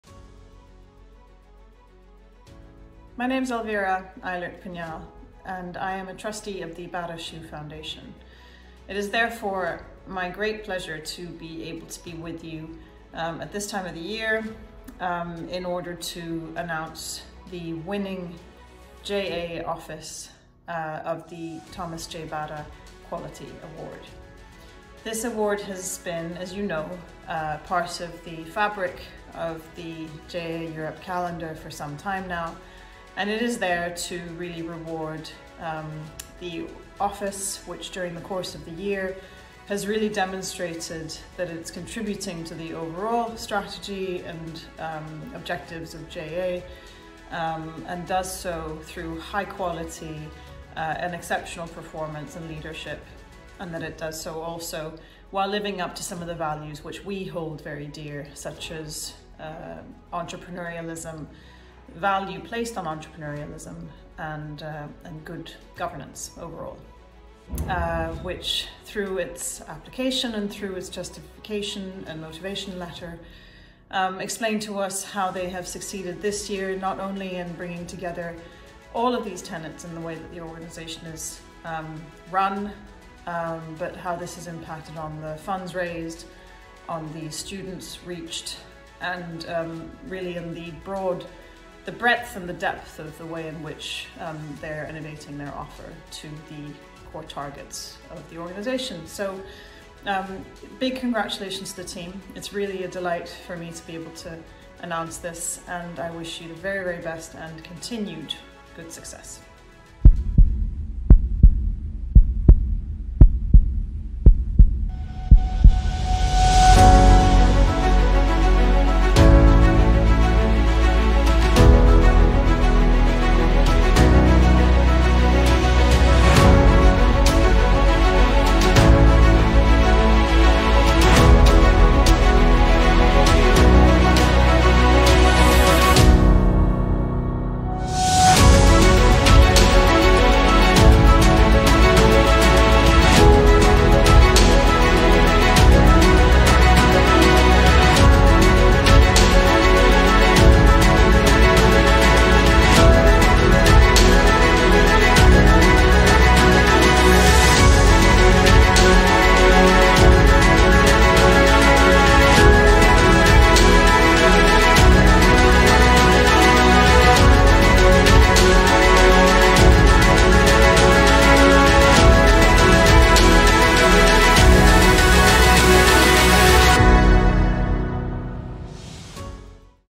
Discover the TJ Bata Quality Award Winner 2023
This Award is designed to recognise the member organisation performing long-term at high-quality standards and substantially contributing to achieving JA Europe’s strategic goal!

Watch the video to discover the winner and hear the message from Elvira Eilert Pignal, Trustee of Bata Shoe Foundation.